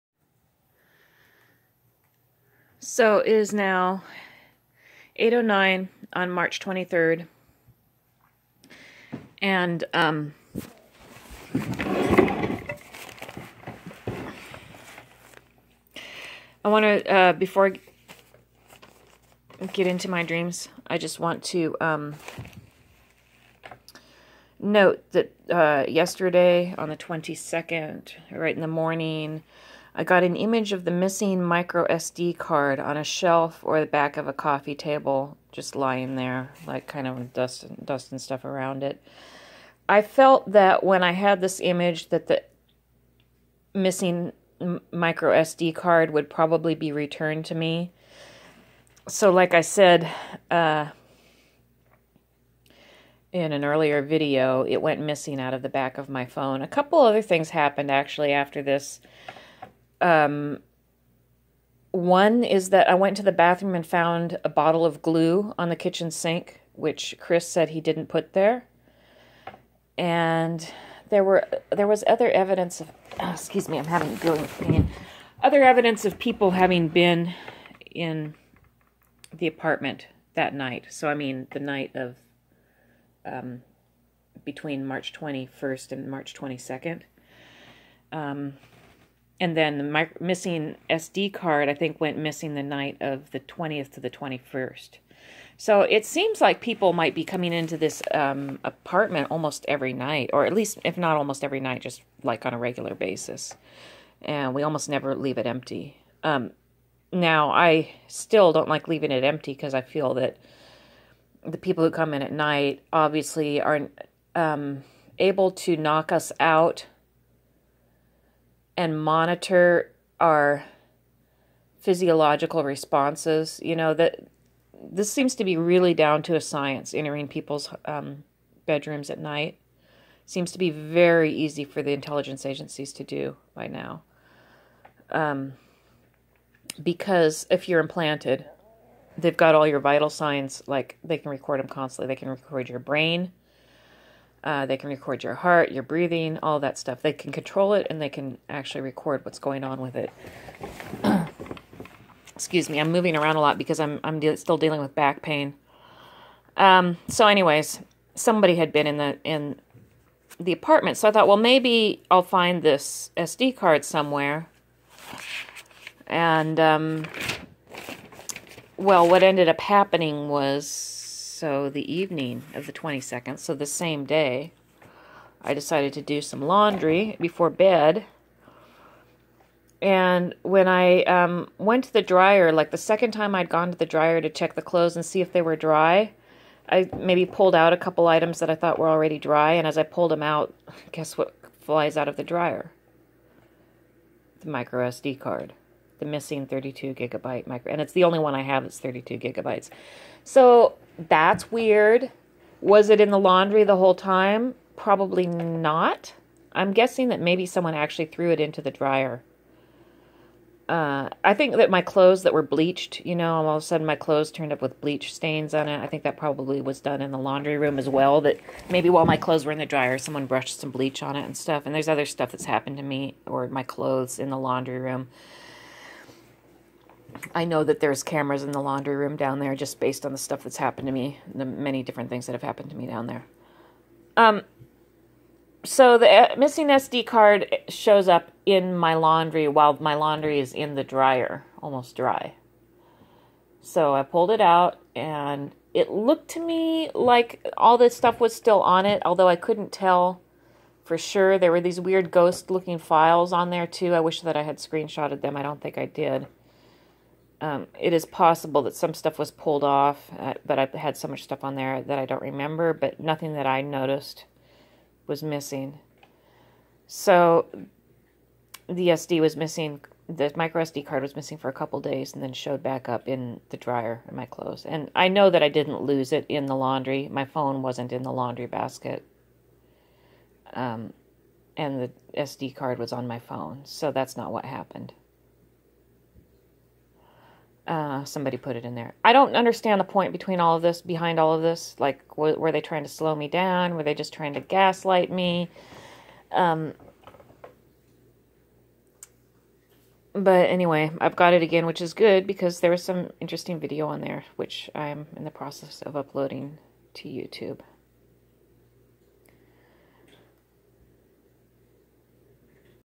March 23: Missing Micro SD card
A full 32G Micro SD card disappeared from my phone during the night of March 21. On the night of March 22 I had a dream vision in which I saw the SD card sitting on a shelf or end table. In the evening of March 23 I found the SD card mixed in with my clothes in the dryer.